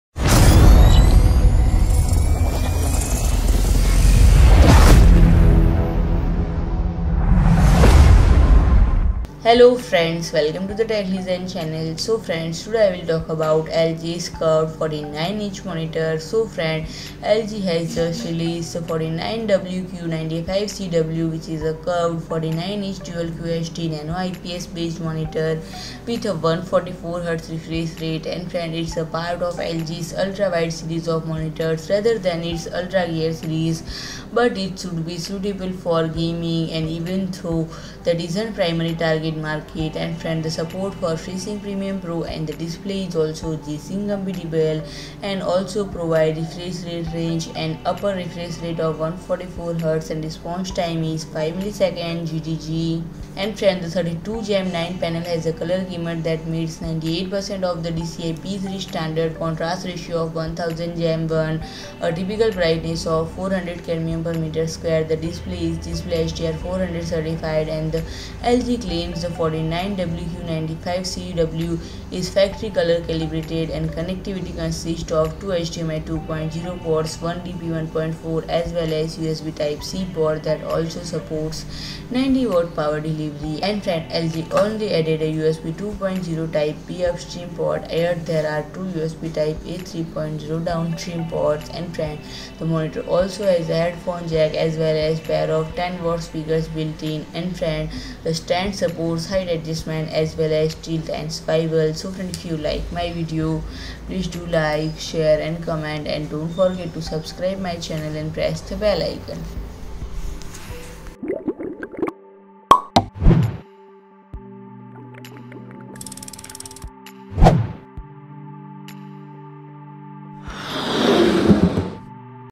LG 49WQ95C-W Curved 49-inch Display Launched With 144 Hz Refresh Rate - Explained All Spec, Features
PRODUCT DETAILS & SIMILAR PRODUCT ORDER FROM AMAZON
SIMILAR PRODUCT :

LG has just released the 49WQ95C-W, which is a curved 49-inch dual QHD Nano IPS based monitor with a 144 Hz refresh rate. It's part of LG's UltraWide series of monitors rather than its UltraGear series, but it should still be suitable for gaming, even though that isn't its primary target market. There's support for FreeSync Premium Pro and the display is also G-Sync compatible, although LG doesn't provide a refresh rate range, only the upper refresh rate of 144 Hz. The response time is 5 ms GtG. LG has also thrown in some of its more gamer focused features such as dynamic action sync which reduces input lag, black stabilizer and a crosshair function. The 32:9 panel has a colour gamut that meets 98 percent of the DCI-P3 standard, a contrast ratio of 1000:1 and a typical brightness of 400 cd/m². The panel is DisplayHDR 400 certified and LG claims the 49WQ95C-W is factory colour calibrated. Connectivity consists of two HDMI 2.0 ports, one DP 1.4, as well as a USB Type-C port that also supports 90 W power delivery. For some odd reason, LG only added a USB 2.0 Type-B upstreams port, yet there are two USB Type-A 3.0 downstreams ports. The monitor also has a headphone jack, as well as a pair of 10 W speakers built in. The stand supports height adjustment, as well as tilt and swivel. LG didn't provide any official pricing.